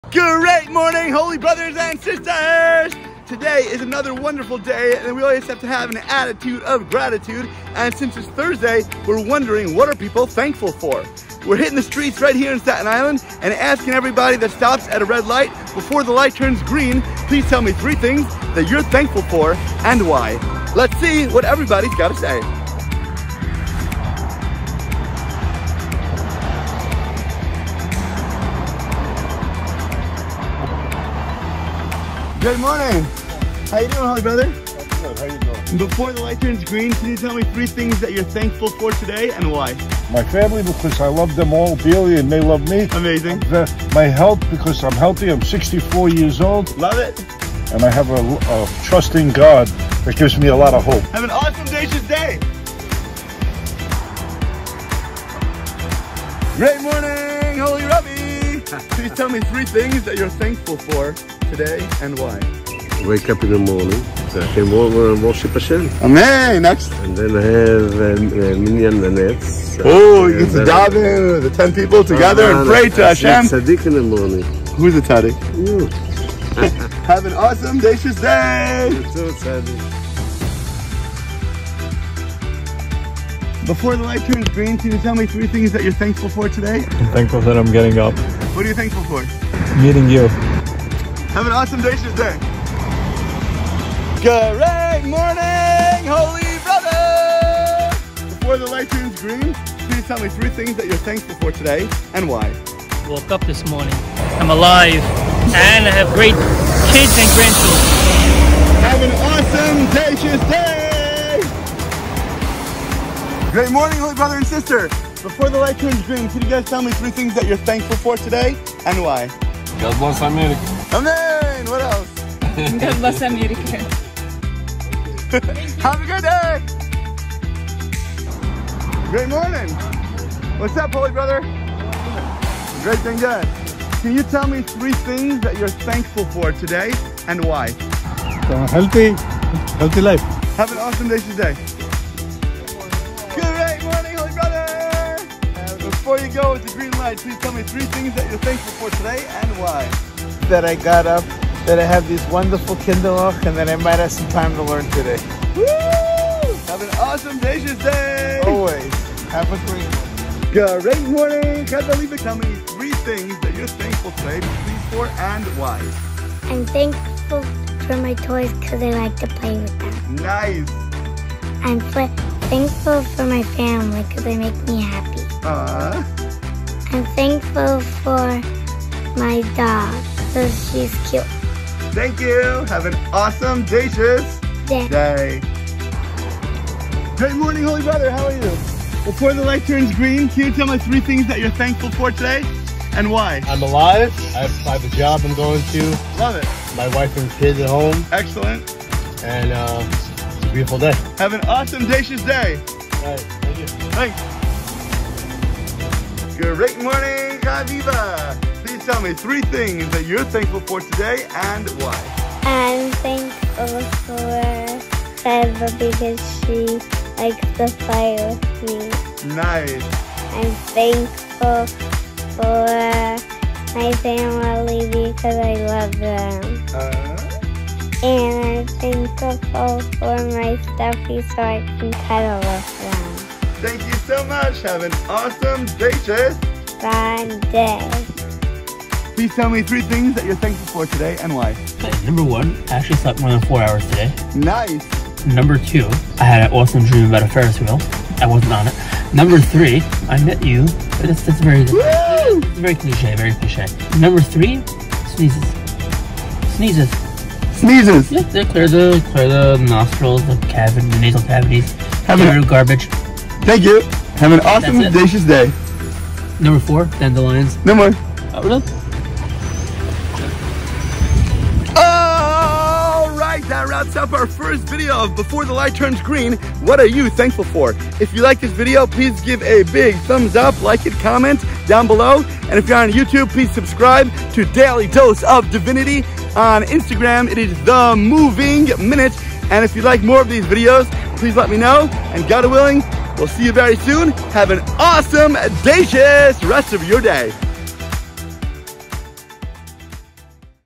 Thankful Thoughts - EPISODE #1 Find out from others what they are thankful for today and why!
We stopped random cars at red traffic lights to ask them the top three things they are most thankful for that day and why! Checkout what everyone had to say in this first episode of the new hit series:
Thursday's THANKFUL THOUGHTS!